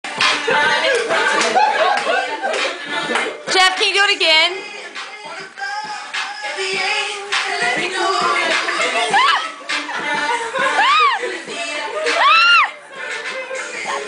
Dancing Banana Crypt Walk
Lil Jeff doing the Crypt Walk!